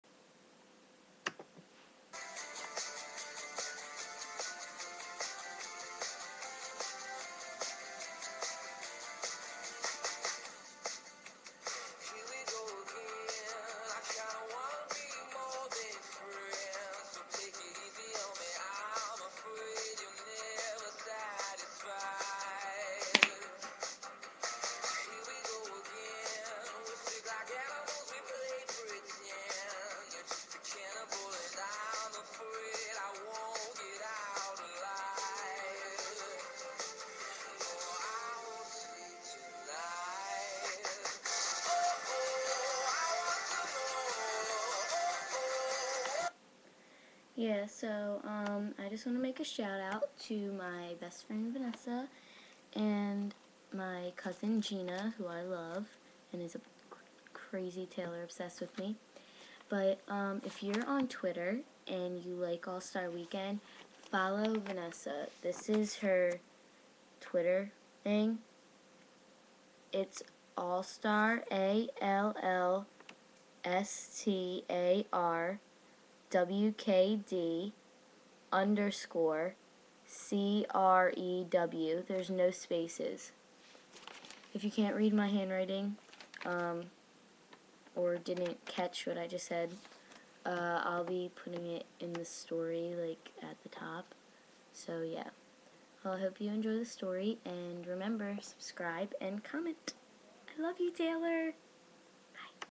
your mine, i'm yours ep.18
the twitter account is AllStarWKD_Crew

NICOLE'S POV:
It's wednesday (th day nathan leaves for L.A) and school just started and i can't wiate to get out of here.we are in algebra and me, taylor and ness are at the same table. me and taylor are holding ands under the table and vanessa is sitting with her head in her hands all the way at the end of the table.
-*in a wisper* ness it's okay you will be able to visit him
ness-easy for you to say, your boyfriend is staying here with you
-yea about that...
ness-what?
-. i'll tell you after class
*30 min later, we are walking out of class*
ness-okay what is going on
tay- well I-
-no let me. taylor got called to go to L.A in a month to go film breaking dawn
ness- oh...sorry about that
-well we have a plan
ness- ??
-i'm going with him
ness- oh great now your leaving to! *starts to walk away*
-*i grab her arm* my plan is for us to buy a house, and me you,nate, cam, zach, taylor, and mikey all live in it in L.A then we convince our parents to let us do it, and we will just enroll in school there
ness- yea right, my parents will never buy it. have fun in L.A *starts to walk away again*
-*i grab her arm again* no if they say no, we will go without telling them, because you arent gonna leave nate and god knows i'm not leaving taylor
ness-*after a long silence* i'm in
-what?
ness- i said i'm in. my parents will never say yes so we will just go without telling my parents. and when i get there i will tell them so they can't really do much then
-ness thats crazy! your parents will flip out!
ness- i don't care. i want to be with nate. and i have enough money saved to help split the cost of the house
tay- don't you guys worry about the house. me and the guys will take care of it.
-really?
tay- yeah, think of it as a gift
-*i kiss him quickly* awww thank you
ness- okay we will talk about it at dinner tonight. but nathan has to do something so we will go in a seperate car, but we can meet you at "alfredo's" at 6?
-yupp that sounds great!
ness- okay see you then *walks away*
-you better tell nate out plan
taylor- okay i'll call him. but you go to class, i don't want you to be late
-okay see ya there *kiss him*
tay- *pulls me close to him and starts kissing more passionatly*
-not here. but later *flash a quick smile at him*
tay- okay see ya
*end of the day*
-tay can you pick me up at like 5:30?
tay- yea cause i have to go drop something off at the directors house anyway
-okay so see ya then
tay- okay i love you *kisses me*
-love you too
*go home get dressed, and it's 5:30 and i hear a knock on the door*
-mom can you get it, it's taylor!
mom-*answer's the door* hey taylor come in
tay- hey Mrs. M
mom- nicole will be down in a min. so where are you guys going?
tay- out to "alfredo's" with nathan and vanessa
mom-okay thats nice but be home by 9:30
-okay mom!
mom- alright love you have fun!
-love you too bye
*walk to taylors lambergini*
- you know you have a sweet car!
tay- *laughs* i know thanks
we get in the car and get onto the highway and we see a car turned on its side and smoke.
-taylor! that looks like nathans carr pull over!
tay- oh my god it does *pulls over*
he's about to get out and i hear a screech then headlights comming right for us
-NO TAYLOR DON'T!
CRAAASSSSHHHHHHHH!